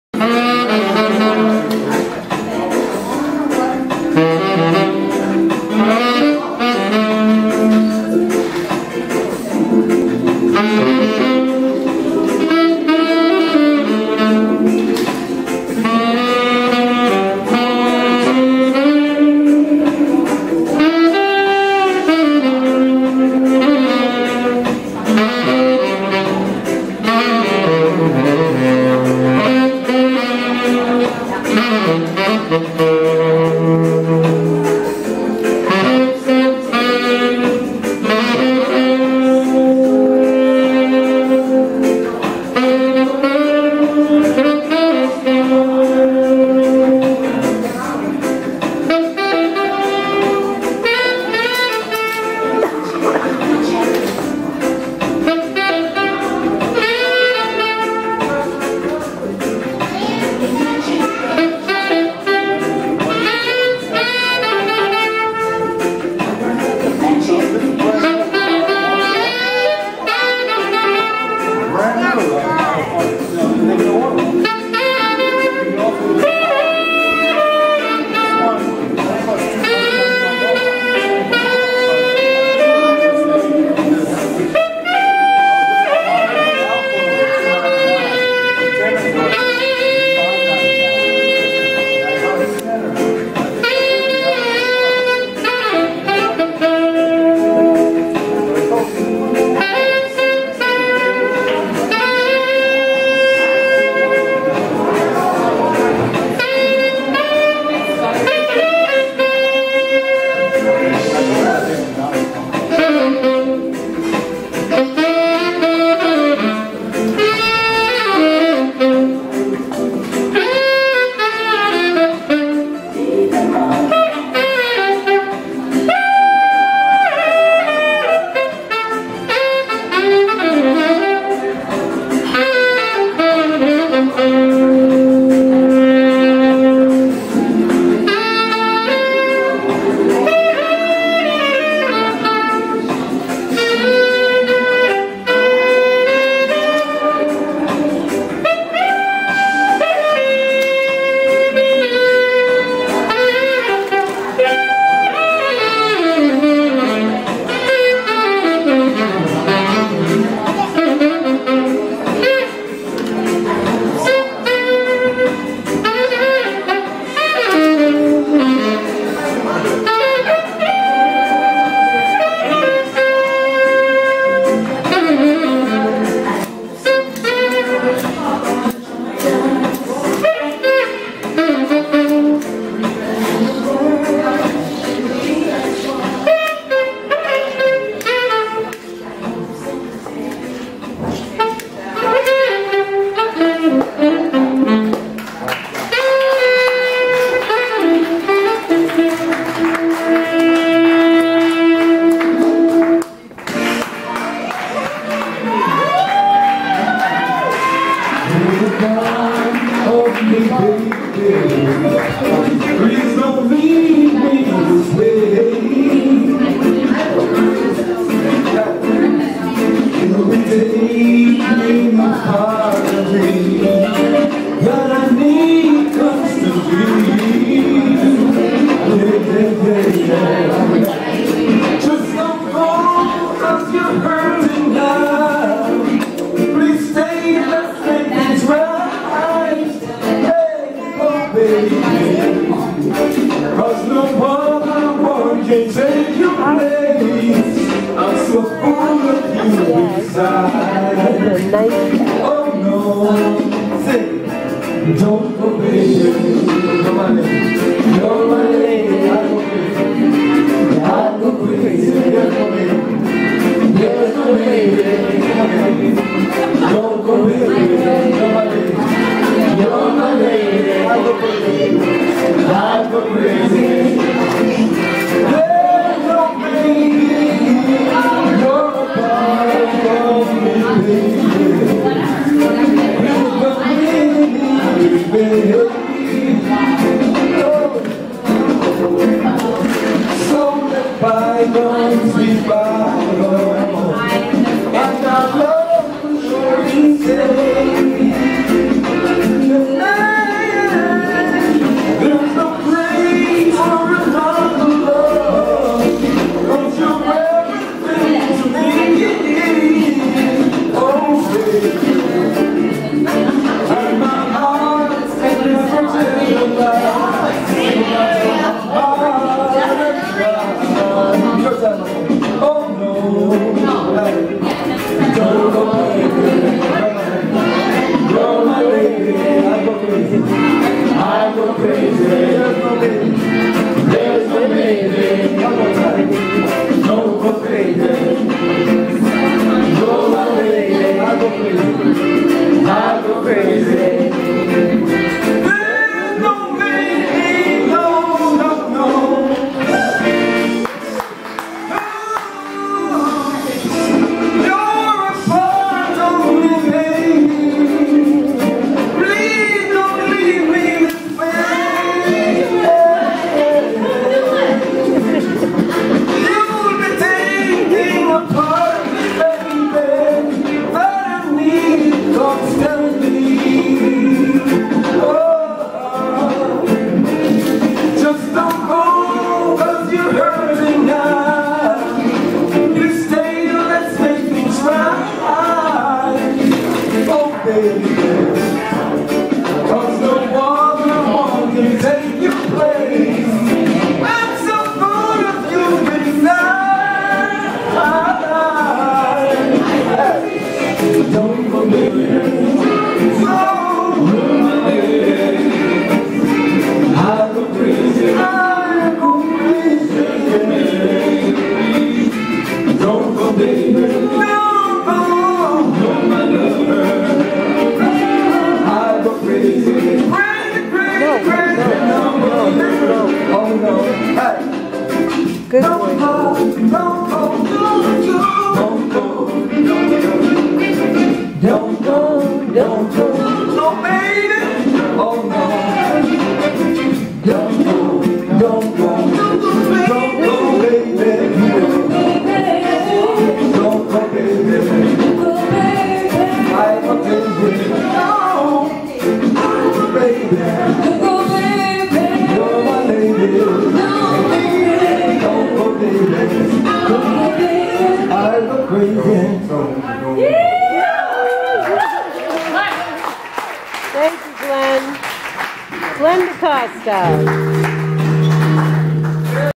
Glen DaCosta
Glen DaCosta sharing some notes at Blue, Congress Street Portland, Maine - summer 2018